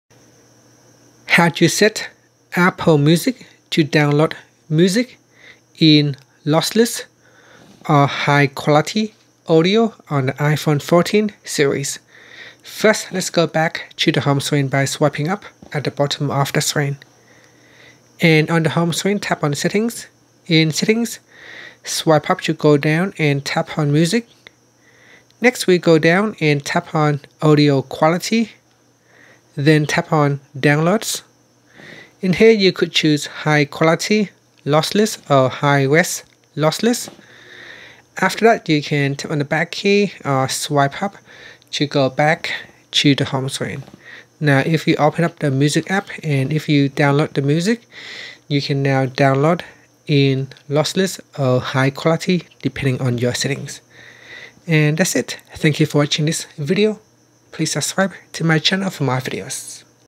iPhone 14's/14 Pro Max: How to Set Apple Music to Download In Lossless or High Quality Audio
Learn how you can set Apple Music to download music in lossless or high quality audio on the iPhone 14/14 Pro/14 Pro Max/Plus.